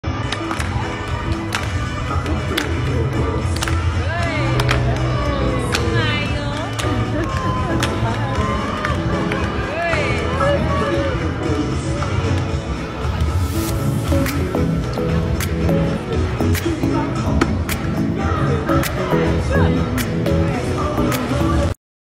Sea Lion funny video | shorts
Sea lion entertains those who come to the water park